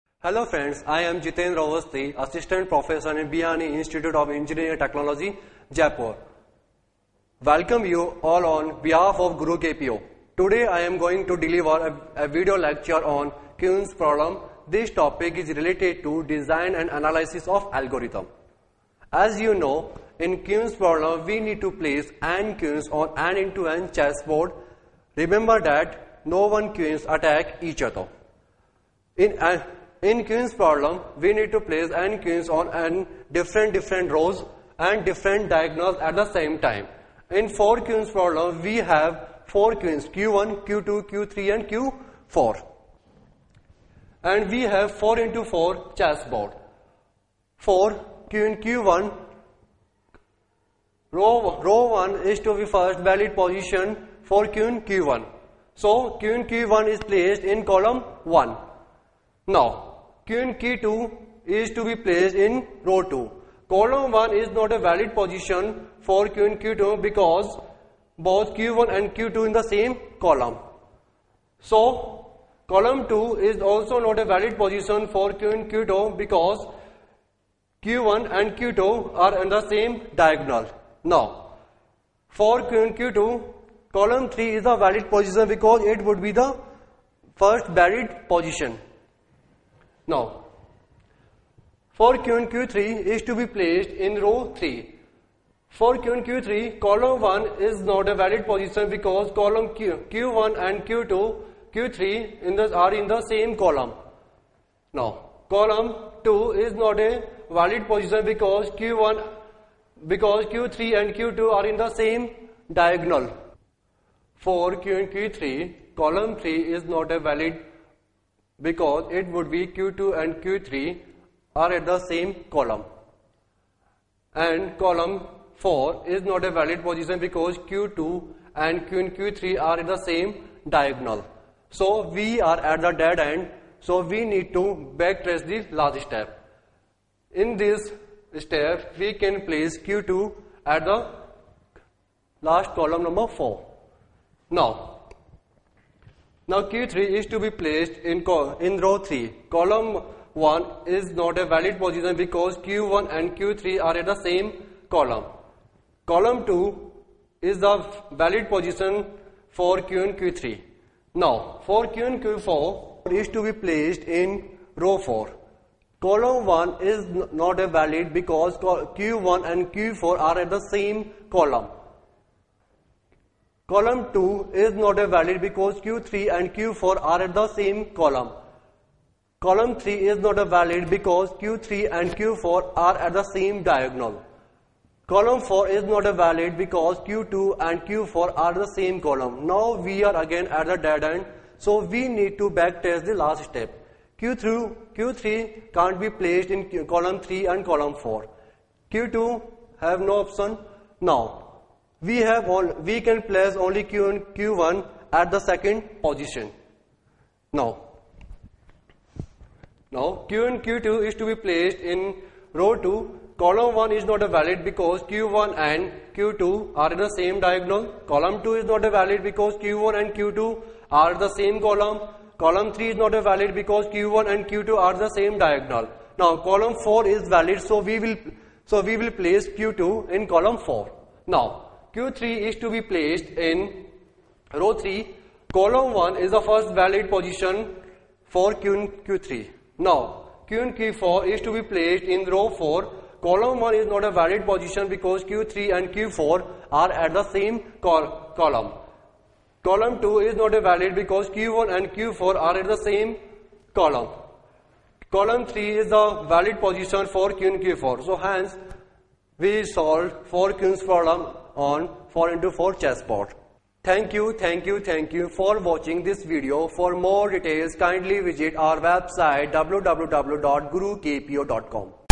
Queens Problem lecture( BTech) by Jitendra Avasthi
This is a part of lecture presented by Mr. JITENDRA AVASTHI, Asst. Professor of B.I.I.E.T. The video is about basic of queens problem & Backtracking, concept.